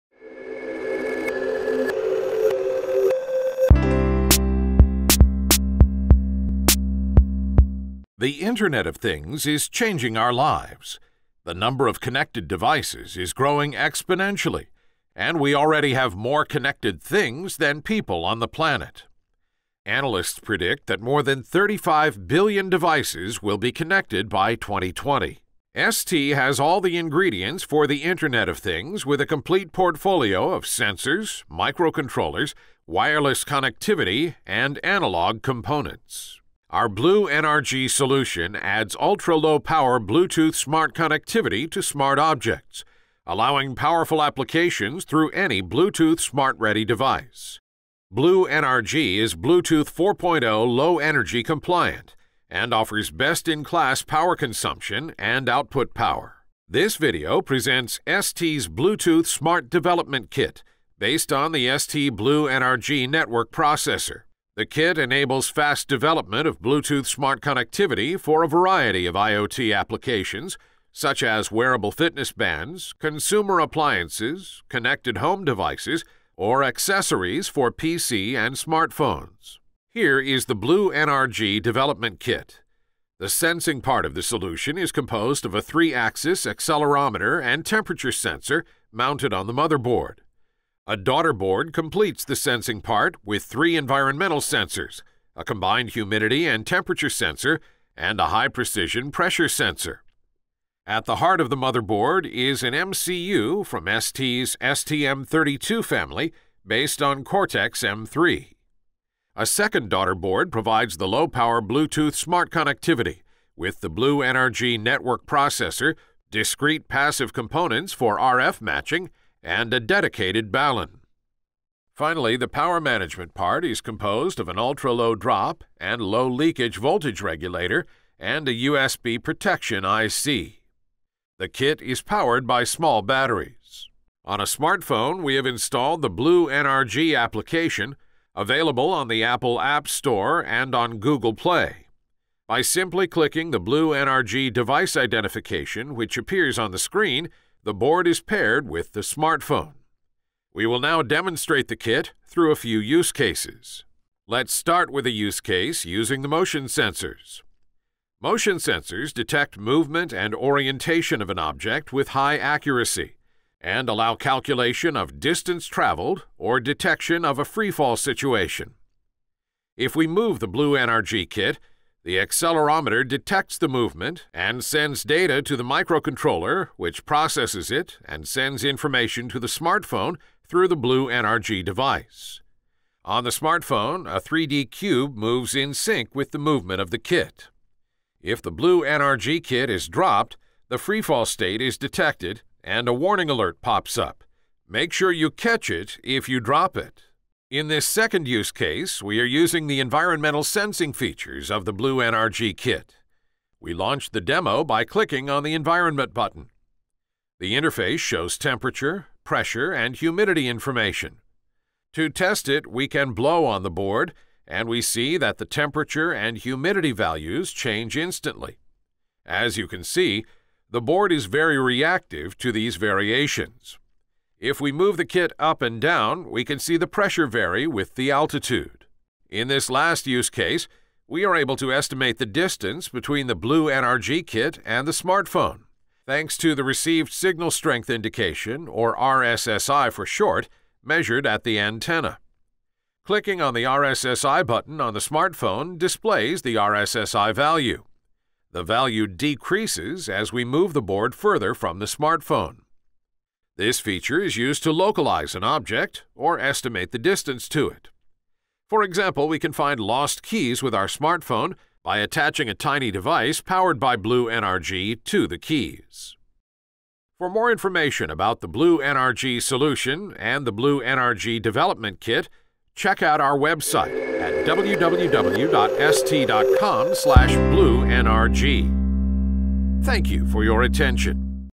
STMicroelectronics STEVAL-IDB002V1 Bluetooth Low Energy demonstration kit
This video is an introduction to the STEVAL-IDB002V1, a Bluetooth Low energy demonstration kit based on the STMicroelectronics's BlueNRG IC.